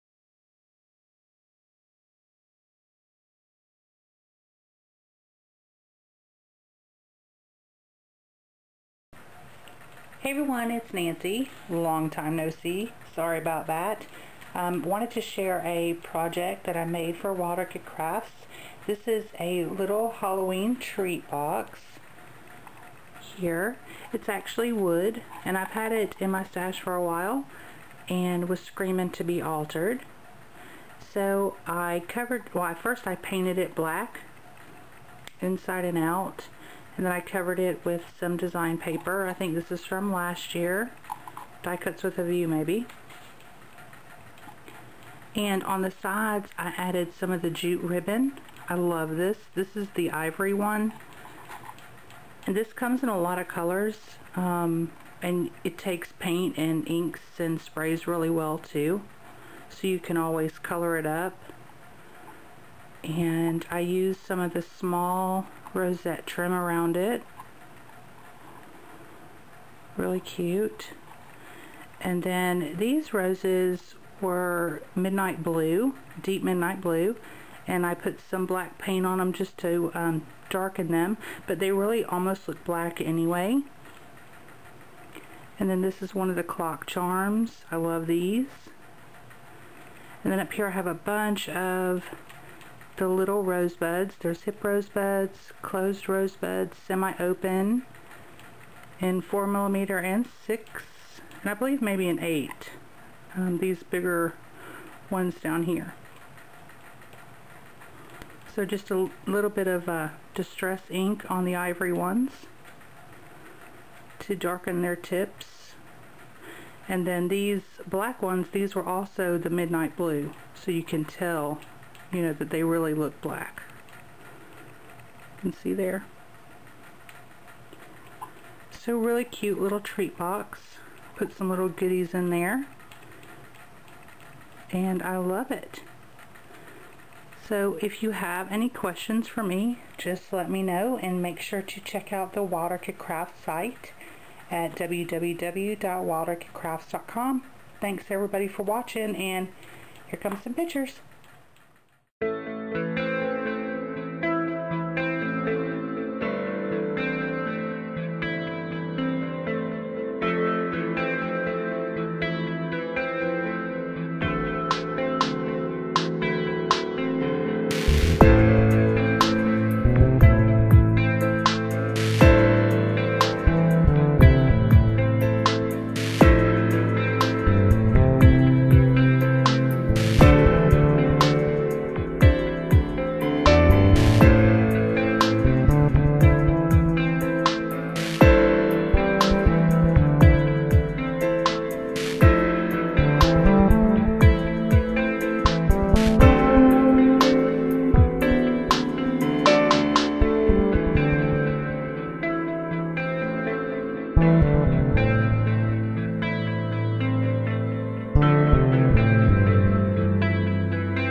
Halloween Treat Bag
***Click "SHOW MORE" for complete details***

LOVE this spooky little Halloween "treat bag".  Its actually wood!  Please check out the WOC site for all of their beautiful items to make your holiday (or any other) project fabulous!

WOC Products Used:
40mm Deep Midnight Blue Tea Roses FM-00364
25mm Deep Midnight Blue Wild Roses FM-00373
Deep Ivory HIP Rosebuds BUD-125
4mm Deep Midnight Blue Closed Rosebuds BUD-035
8mm Ivory Semi-Open Rosebuds BUD-096
4mm Ivory Closed Rosebuds BUD-014
6mm Midnight Blue Closed Rosebuds FM-00328
Black Small Organza Rosette Trim OZ-00017
Cream Jute Ribbon JUT-001
Metal Clock Charms JW-00024